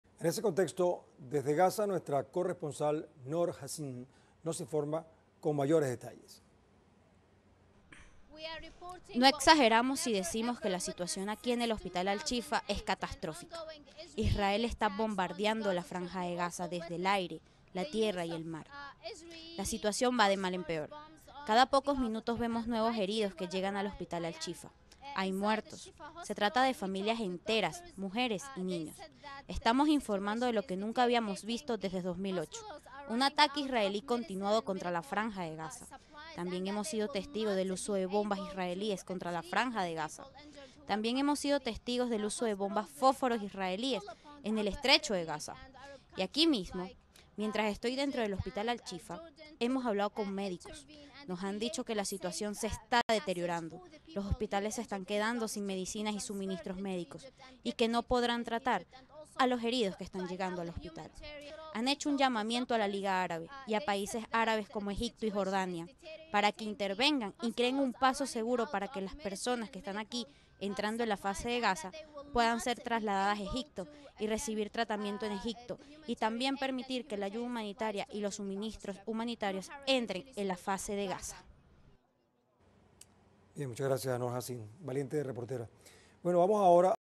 Empeora situación en el hospital Al-Shifa luego de arremetida israelí
Israel intensifica bombardeos en la Franja de Gaza y familias enteras heridas llegan al centro de salud palestino, el cual está colapsado y se agotan los suministros médicos y medicinas en el hospital. teleSUR